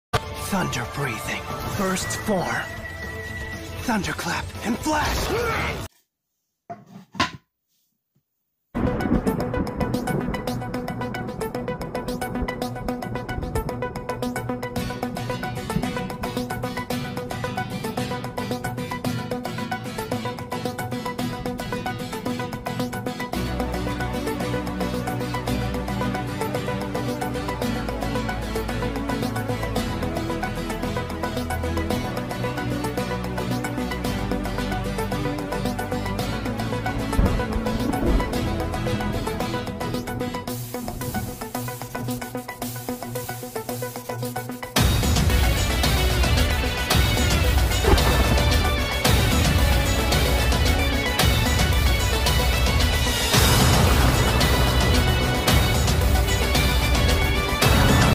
How to make a wooden Zenitsu Plaque (Demon Slayer) - Woodburning 1
How to make a wooden plaque of Zenitsu from Demon Slayer this was a custom for a friend I'm going to be doing more :).